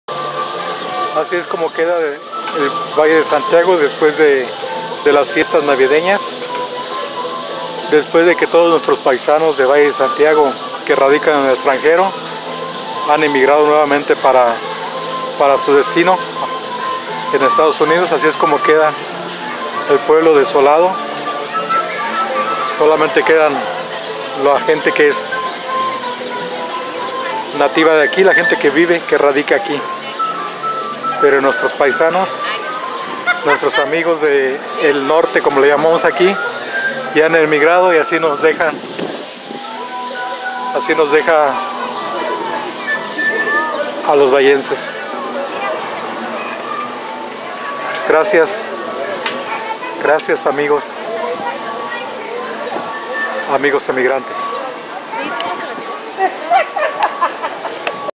El jardin de Valle de Sgo se quedo sin los paisanos
Cada ano llegan de USA a Valle de Santiago,Gto,Mex nuestros paisanos y aqui les mostramos como queda despues que regresan al norte, el jardin queda desolado.